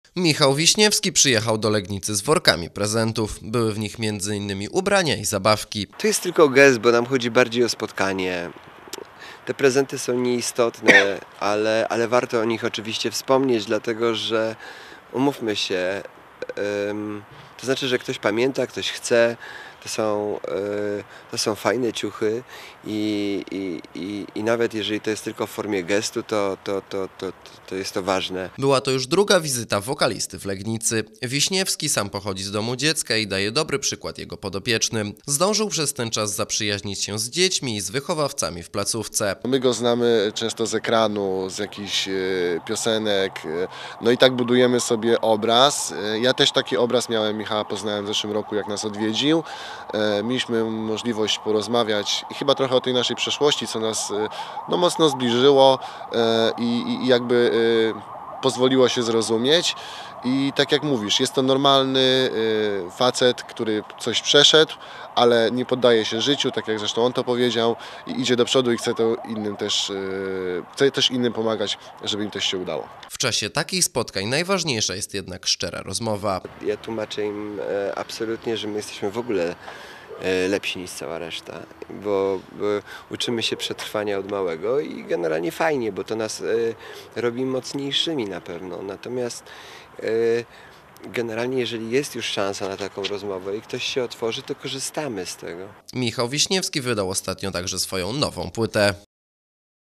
Wiśniewski odwiedził podopiecznych legnickiego Domu Dziecka
Po raz drugi w Domu Dziecka przy ul. Wandy pojawił się Michał Wiśniewski. Znany wokalista rozdał prezenty i rozmawiał z podopiecznymi placówki.